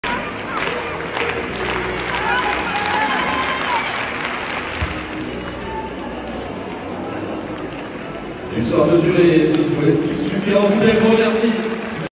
Maëlly Dalmat est Championne de France Élite ! 🔥🔥🏆🏆
Maëlly Dalmat est Championne de France Élite en salle de saut en longueur avec un bond à 6,52 m !
1re performance française de l’année, 4e européenne, 15e de tous les temps en France...